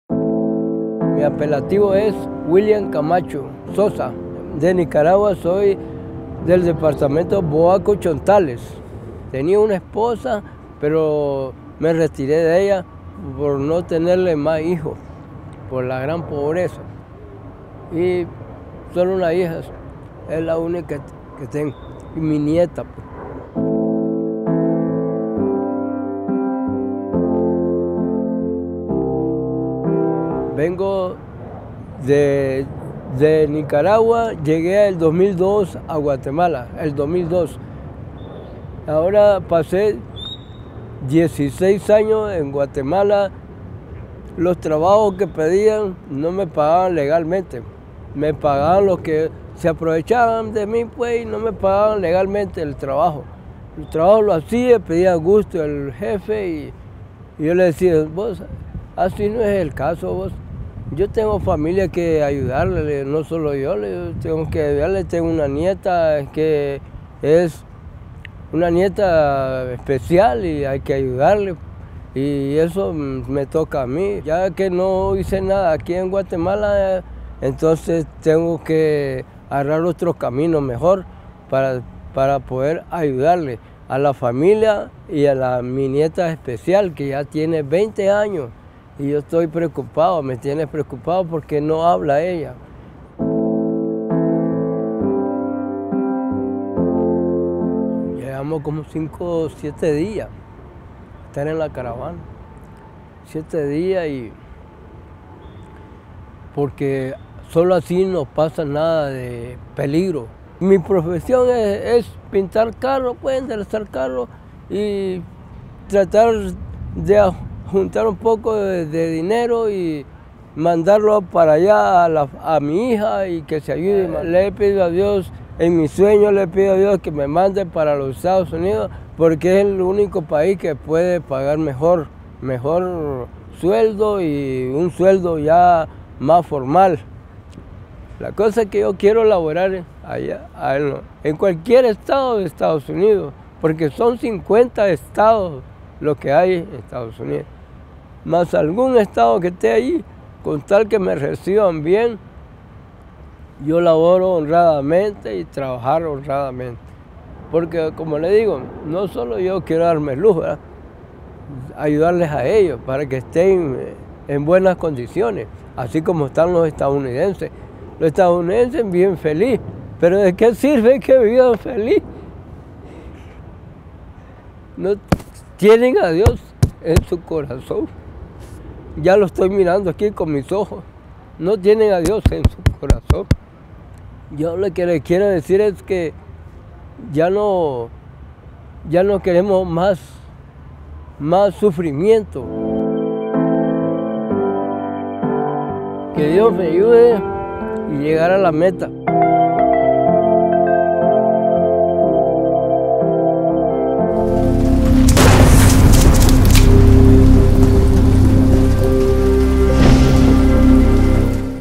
William Camacho, huir de la pobreza para salvar a la familia
William Camacho Sosa es un hombre de edad avanzada que se integró a la caravana del éxodo migrante en Guatemala. Él busca llegar a Estados Unidos para poder seguir trabajando y ayudar a su nieta.
¡Historias de vida, sólo en Rompeviento TV!